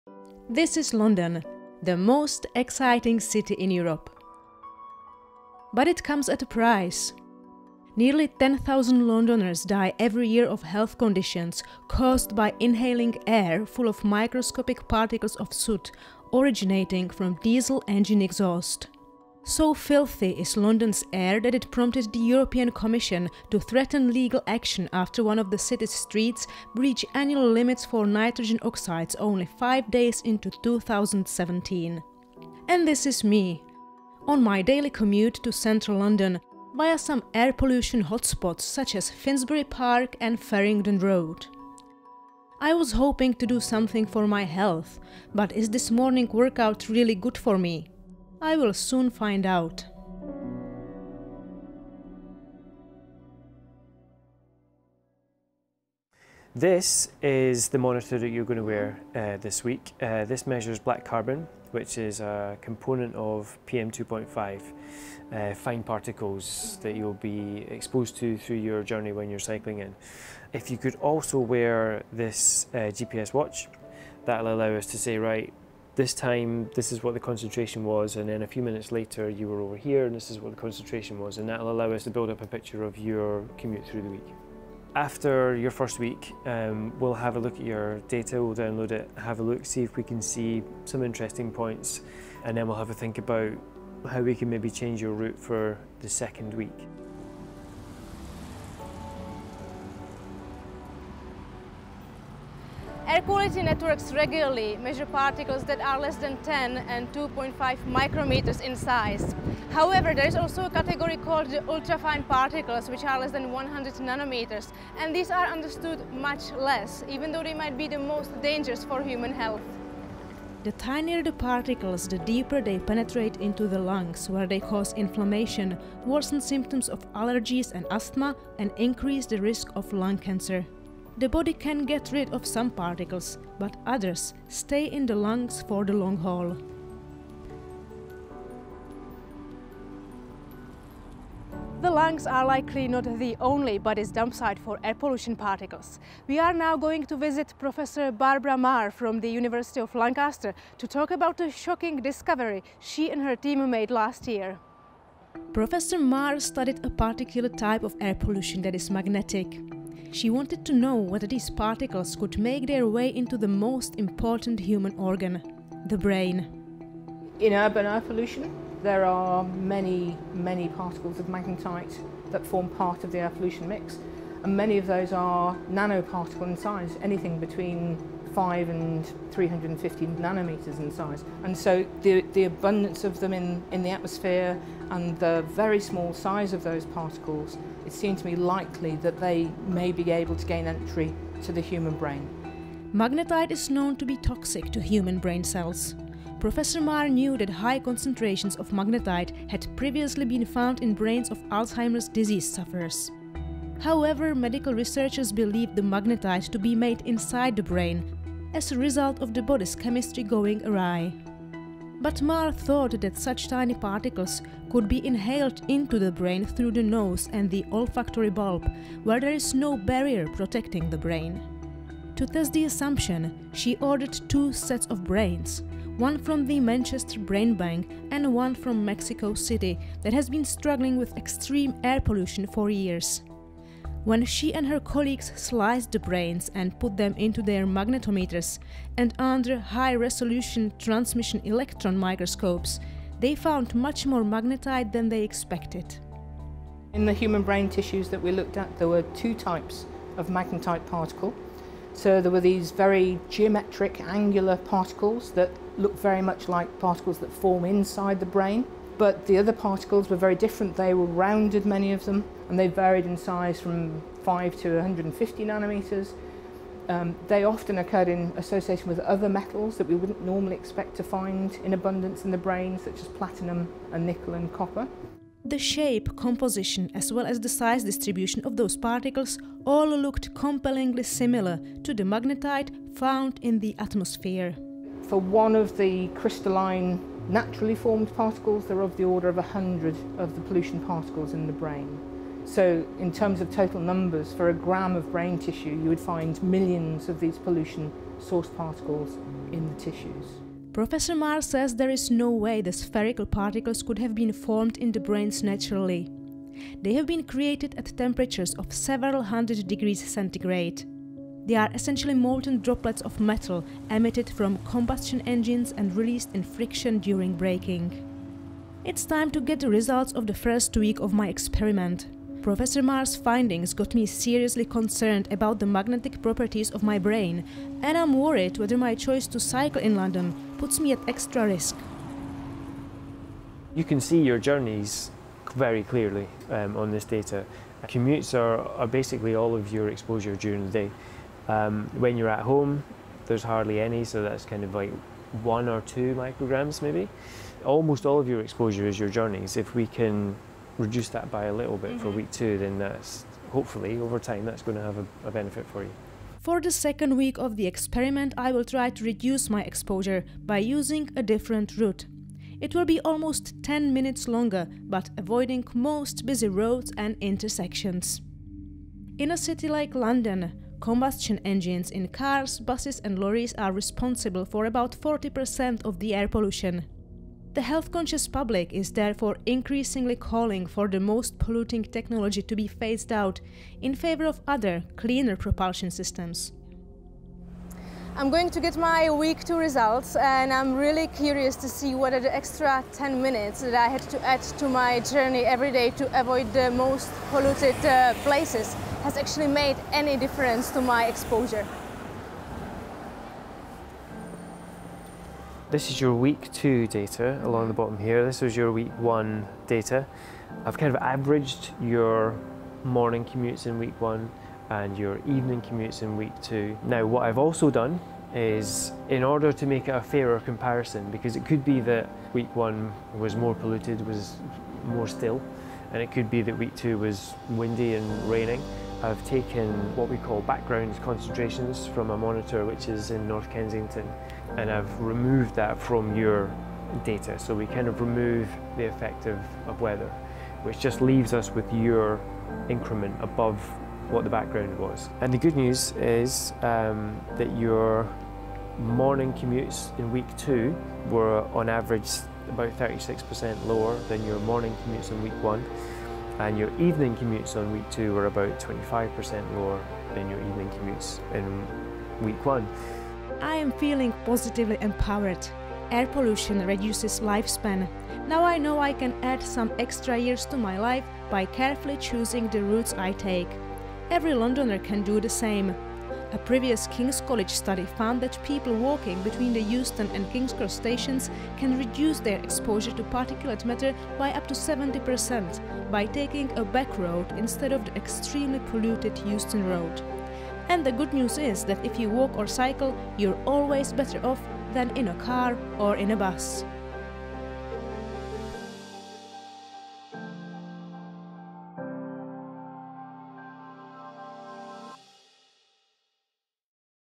Cycling Pollution: How safe is my commute?
Air Pollution is a very real danger, especially in major cities. In London, 9,500 people are estimated to die every year due to health complications related to exposure to air pollution.  The notoriously bad air in the UK’s capital prompted the European Commission to issue a legal warning shortly after one of the city’s streets breached limits for nitrogen oxides only five days into 2017.  With the amount of pollution exposed to cycling commuters, we ask 'How safe is my commute?'

We follow journalist Tereza Pultarova as she looks at what can be done to improve her daily commute by avoiding exposure to the dirtiest air.